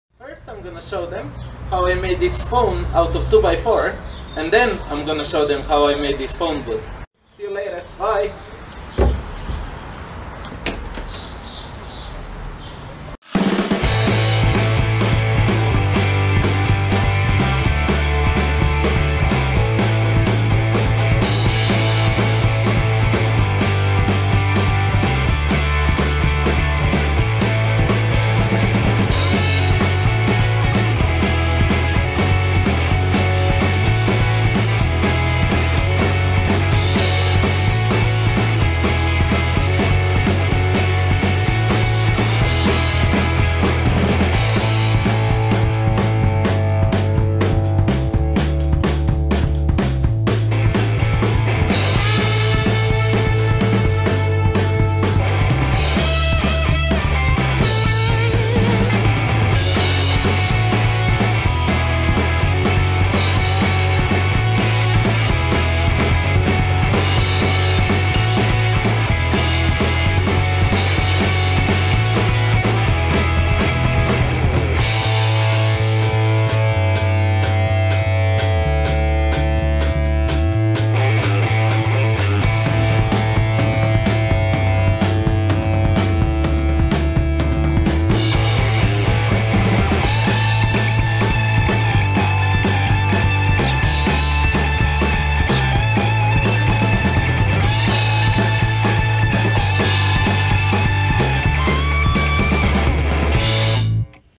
Making the Wooden Antique Phone (2x4 Contest 2015) - Ep 030
I made this wooden phone out of single 2x4...and a circle of MDF.
I had a ton of fun...and did it in 6 hours including recording and editing.
This is for Summer's Woodworking 2x4 Contest 2015.
Thanks Brian!

PS I used MDF for the dial due to lack of time/good weather to cut and glue the pieces out of 2x4.

Page design: Barbara MT (my lovely wife)